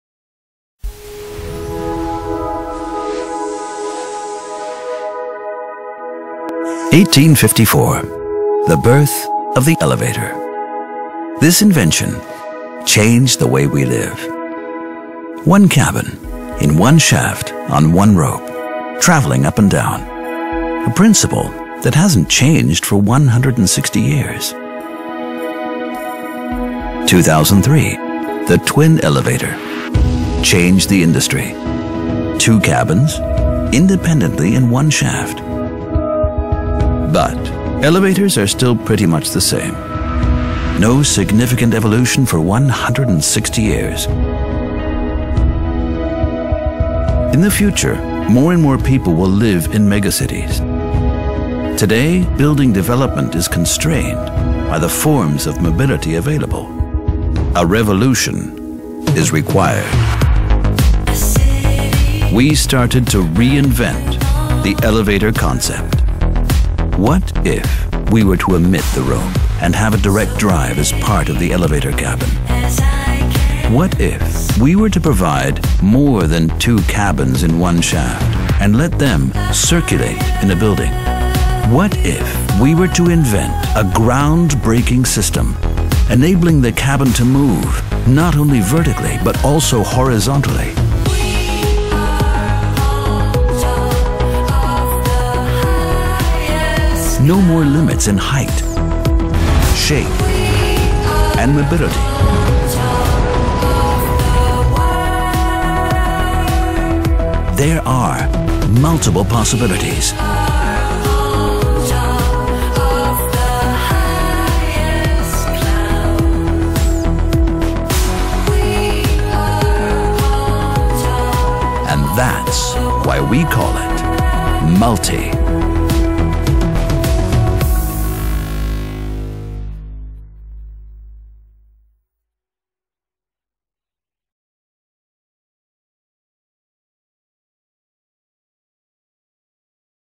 Технологии будущего Магнитные лифты, маглевы в современных здания заменят обычные лифты Инновационн
Привычные нам лифты скоро уйдут в небытие. На смену им придут маглев лифты на магнитной подушке. Магнитный лифт позволит увеличить пропускную способность и скорость перемещения пассажиров и грузов.